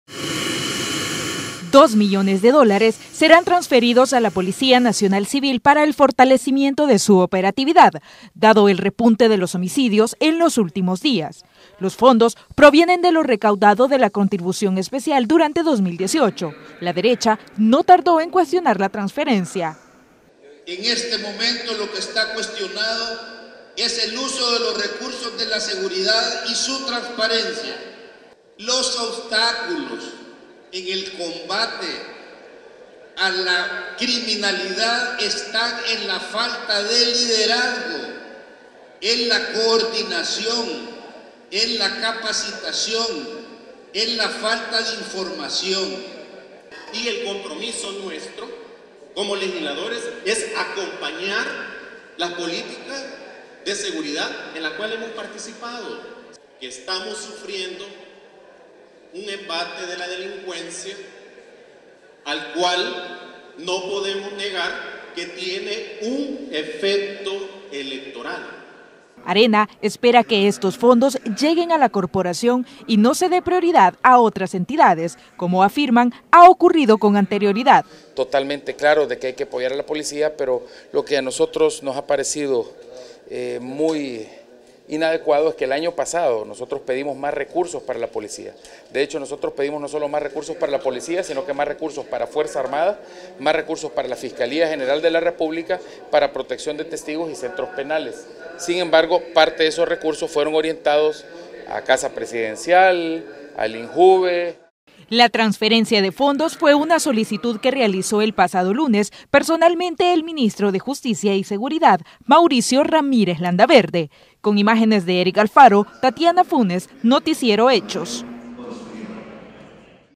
Aprueban fondos para fortalecer a la corporación policial
Con 65 votos fue aprobada una reforma a la ley del presupuesto para dotar de más recursos a la PNC, tras los recientes ataques en contra de elementos policiales.